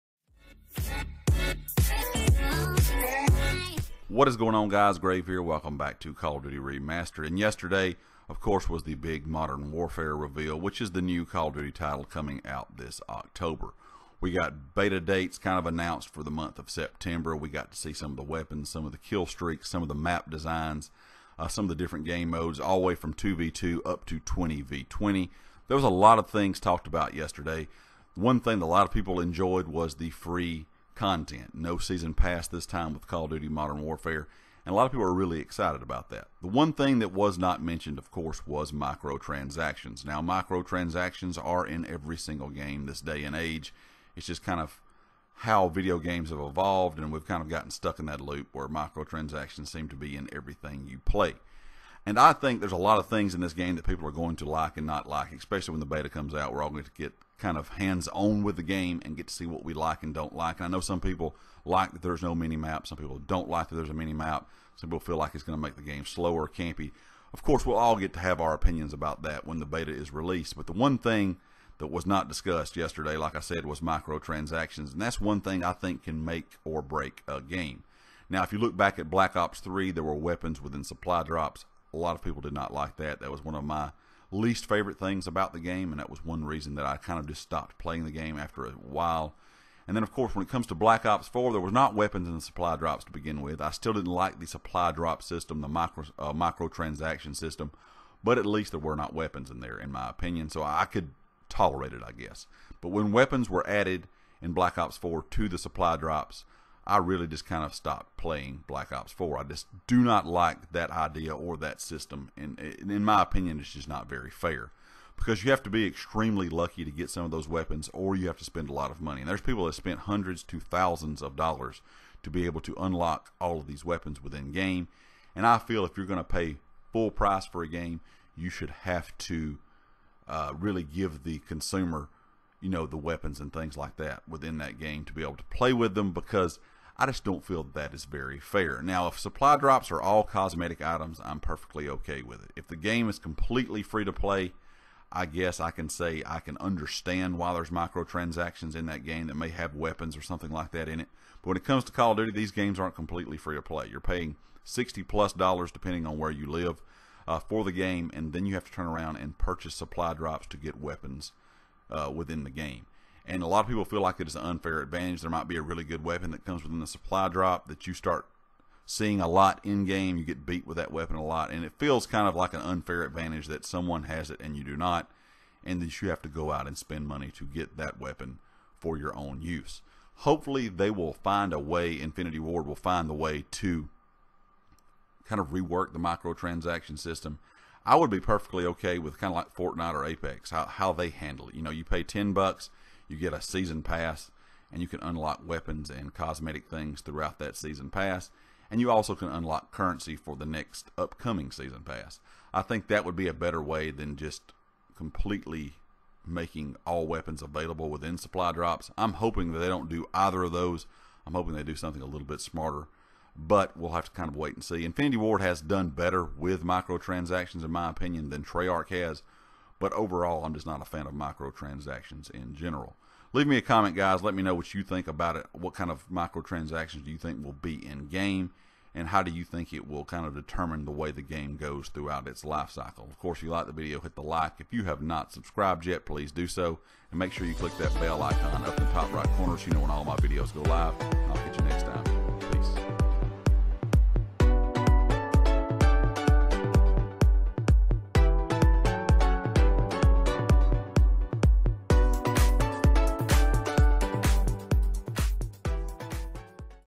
Call Of Duty: Modern Warfare Will The Game Have Microtransactions?
If modern warfare has microtransactions this could be what makes or breaks the game. What are your thoughts?